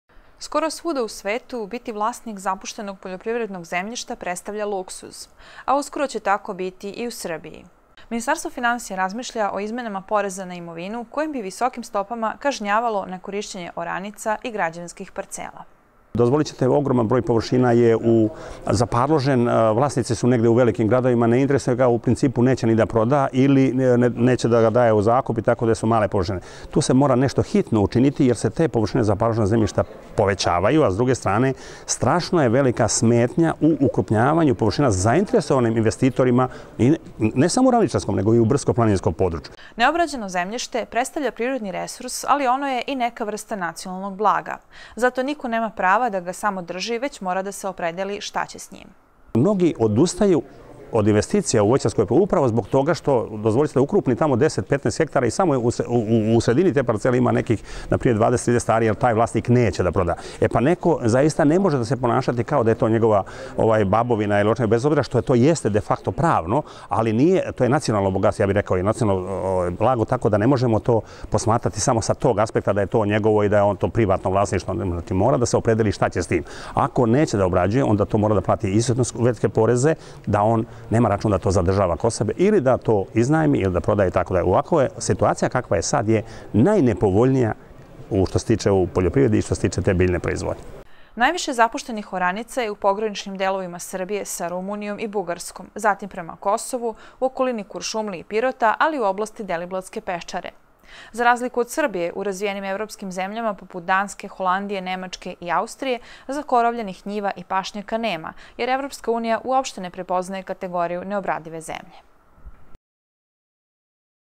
Zapušteno imanje -- luksuz
U okviru poreske reforme biće uvedeno i oporezivanje nekorišćenog poljoprivrednog i gradskog zemljišta. Zbog velikih površina neobrađivanog zemljišta Srbija gubi ogromne prihode. Prema podacima Uprave za zemljište Ministarstva poljoprivrede, svake godine ostane neobrađeno od 200 do 350 hiljada hektara oranica. Za razliku od Srbije, u razvijenim evropskim zemljama nema zakorovljenih njiva i pašnjaka.